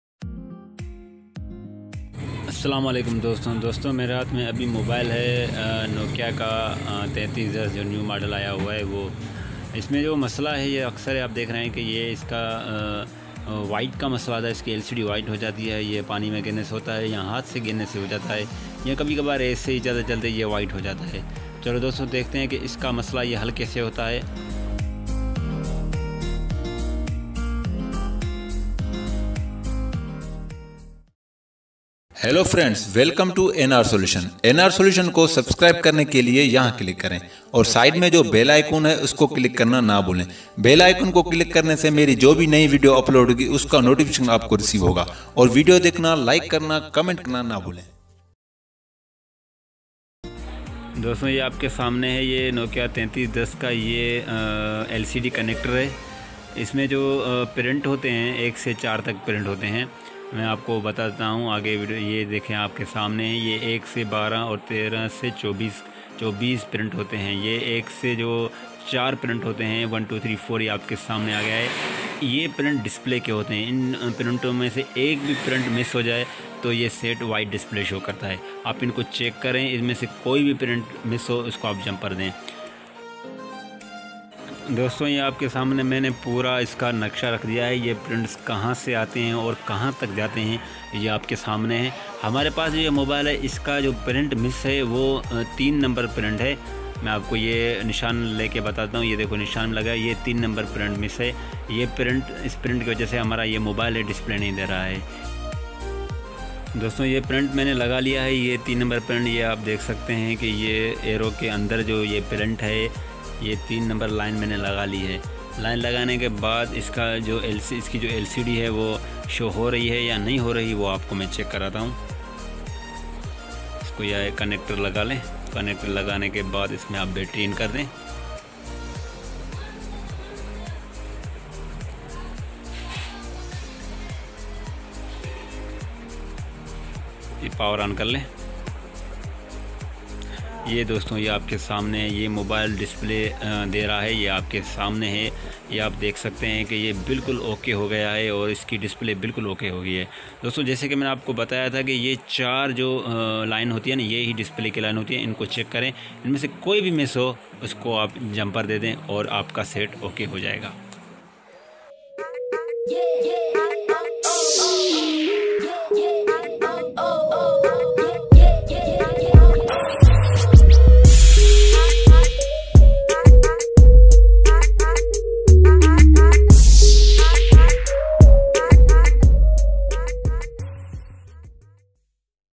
Nokia 3310 White Lcd Fix Lcd Ways -Urdu/Hindi
Nokia 3310 Model Lcd White Problem
Missing Some Line Mobile Detected White Display
Applay Jumpers White Display FiX Jumper See To Watch Video And Follow Video 100% Problem Solved